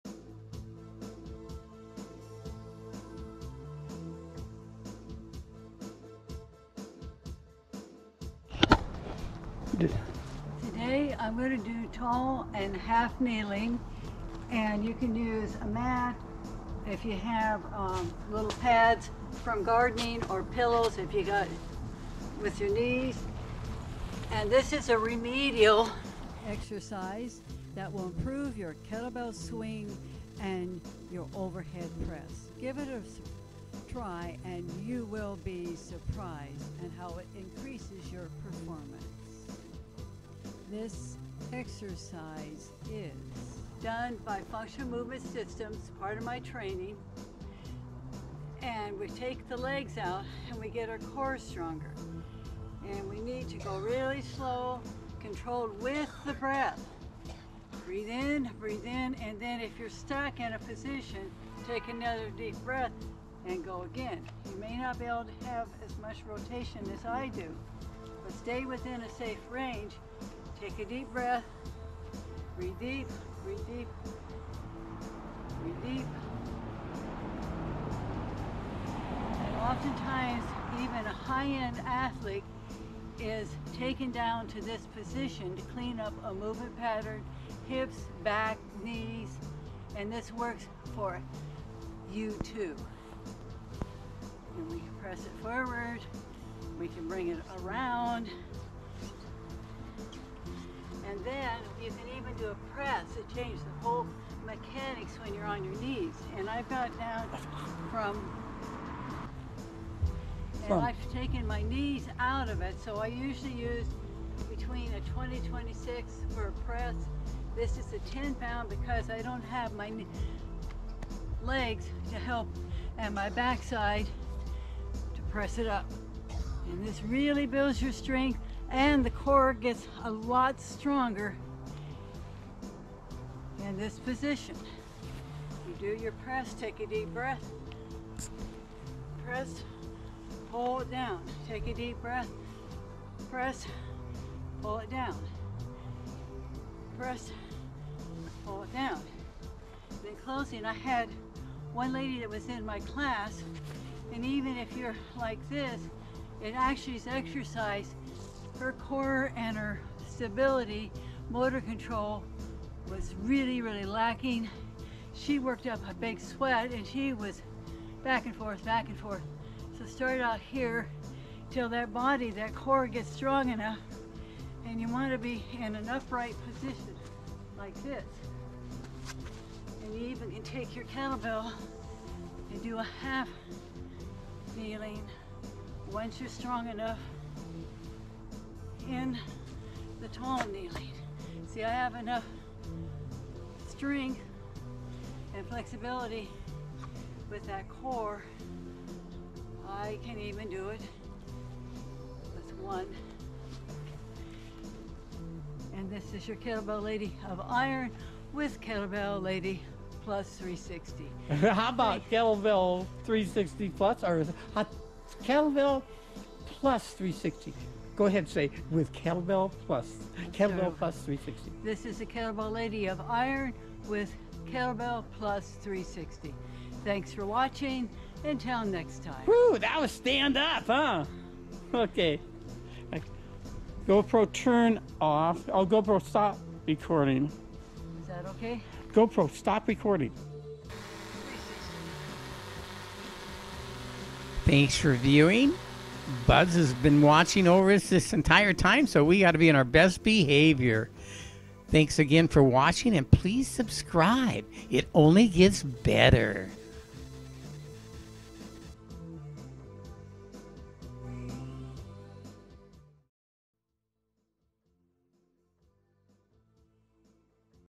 Corrective for Kettlebell Swing & Over Head Press In the Safety Zone
Tall & Half Kneeling   tall-kneeling and half-kneeling are corrective exercises that builds posture & improves the kettlebell swing & press.  Why?  The position is the pattern that sets up the movement for static and dynamic movement. This means you have integrity and efficiency of movement. When you are off your feet mistakes in movement are more apparent working quads, hamstrings, core, and stability. The hips, the spine and the upper thoracic spine are targeted. Vladimir Janda described this Lower Crossed Syndrome making this movement pattern challenging.
How to:
You’ll hold the weight in front of you and as you rotate to the left or right take a deep breath. If you are unable to move any further take a deep breath staying in that position to see if you can rotate a little further once the muscles release tension. When you take the weight behind you with tall posture squeeze your shoulder blades together to help correct rounding shoulders and chicken neck : if you’ve get ‘1’s’ on the shoulder mobility or the active straight leg raise in the Functional Movement Screen, that means you’re bringing a mobility problem to a motor control or stability workout.. Kettlebell Lady Leanne Wylet is a certified Functional Movement Systems administrator. She can be contacted on how to get yourself tested.